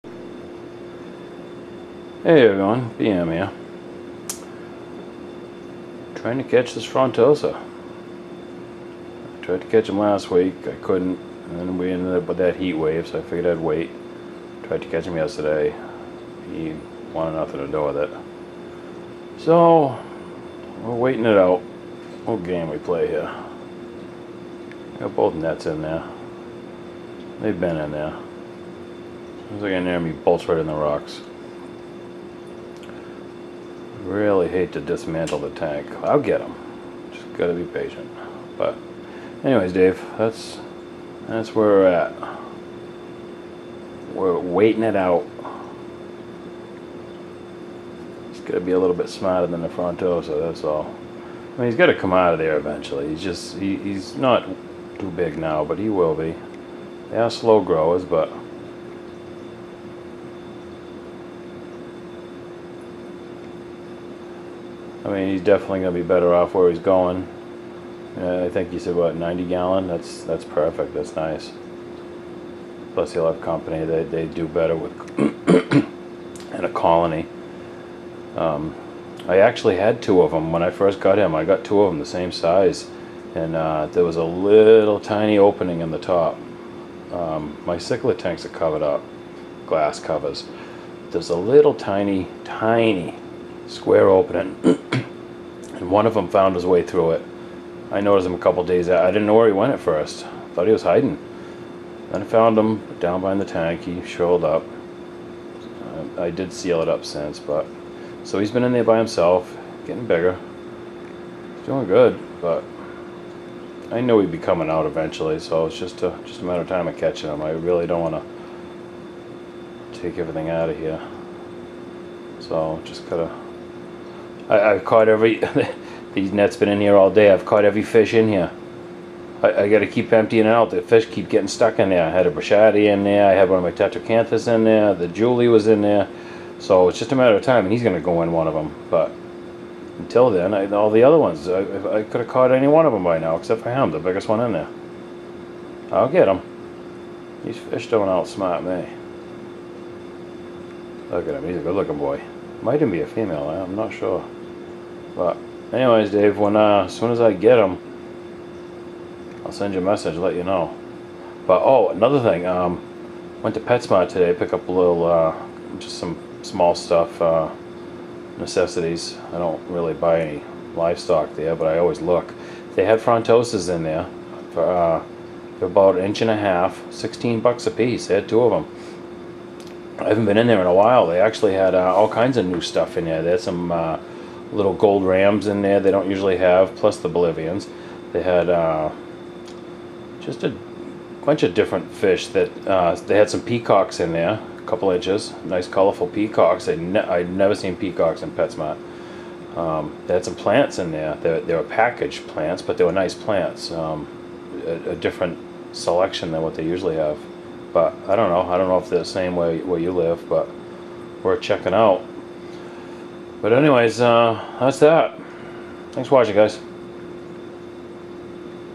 Fishing for a Frontosa
Davidsaquarium is a better home, now i need to catch him
he will be going to a bigger tank and will be with other Frontosas, he will deffinately be happier in the long run. Now i just need to catch the little bugger.